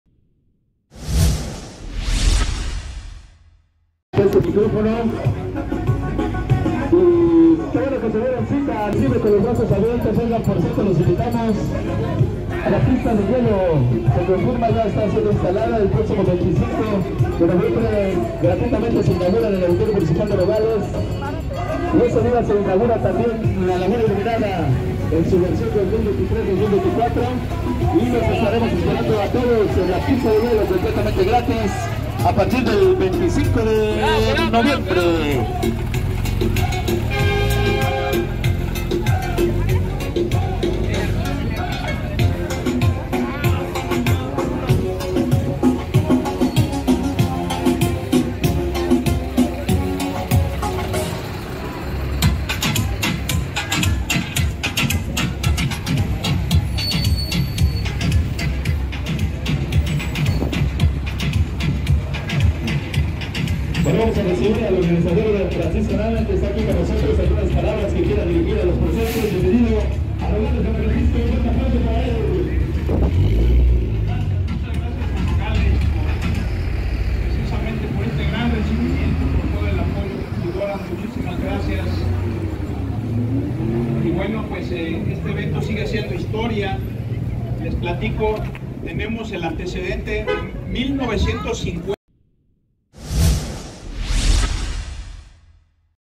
Bólidos del Rally “Del Golfo al Pacífico” pasearon por Veracruz
La edición 22 del Rally “Del Golfo al Pacífico” llegó a la zona centro de Veracruz con 2 paradas, una en Nogales y la otra en Mendoza. En esta ocasión se trata de una carrera con causa que recaudará víveres para los hermanos afectados por Otis en Acapulco.